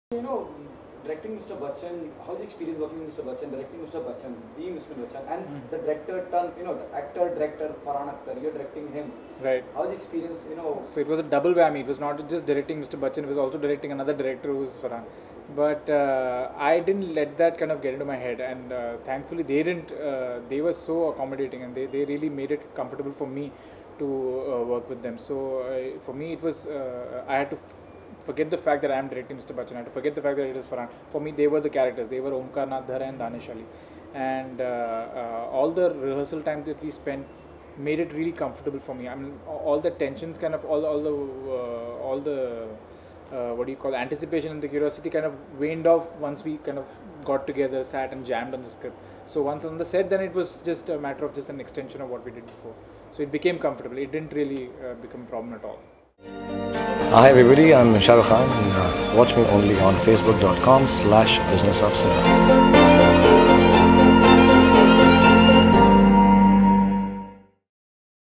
Bejoy Nambiar Shares His Experience On Directing Farhan Akhtar And Amitabh Bachchan!
Wazir director Bejoy Nambiar shares his experience on directing Farhan Akhtar and Amitabh Bachchan!